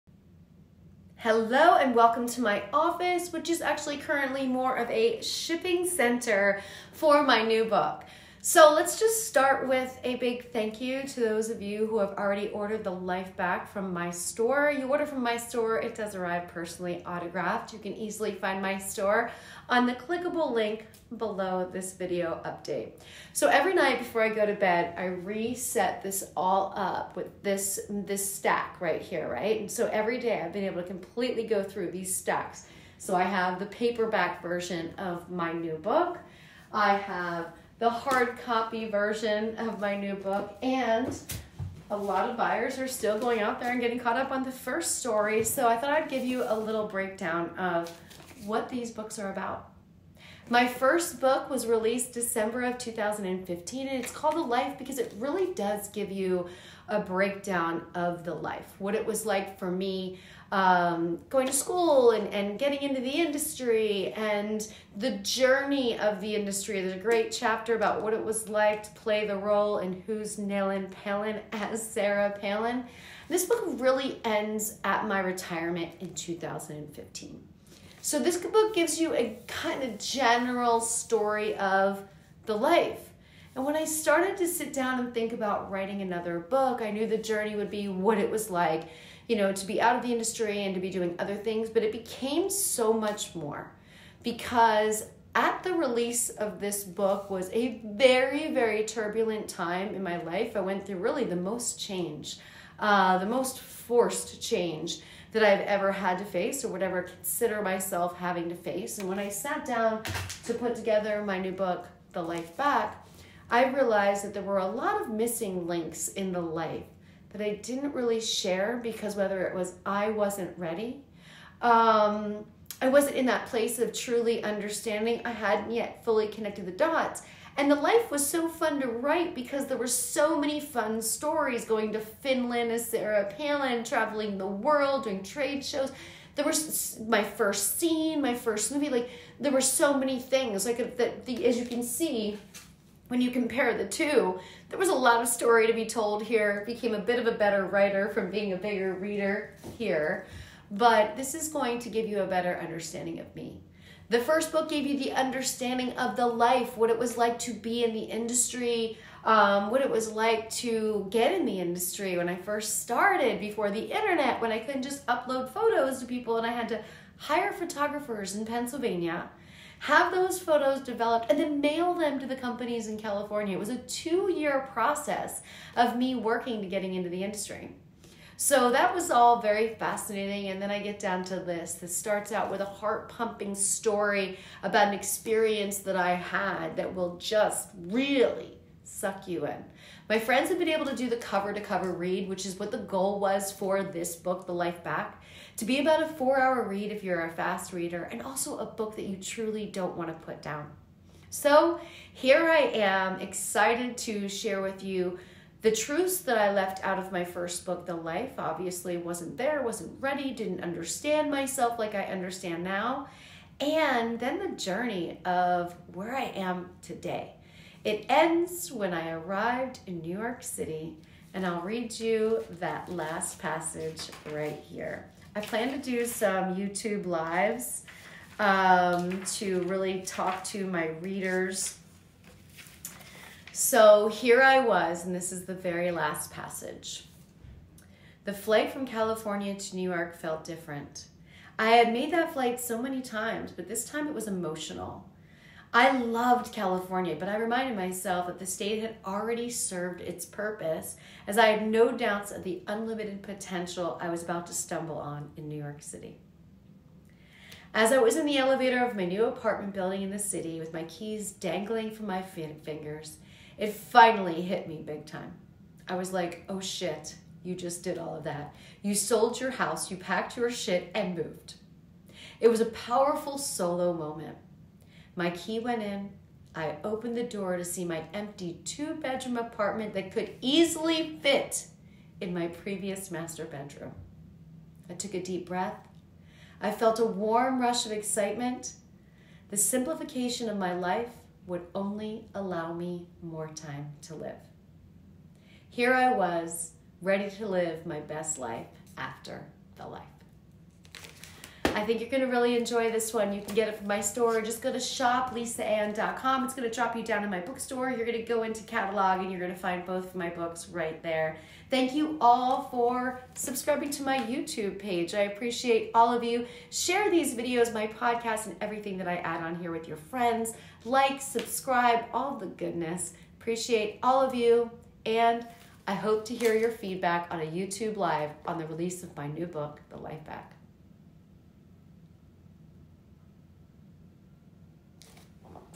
The Life Back ...
My 2nd Book is available and I wanted to share a little back story with you. If you would like a physical copy of The Life Back, personally autographed, I put the links for each version below! Happy Reading!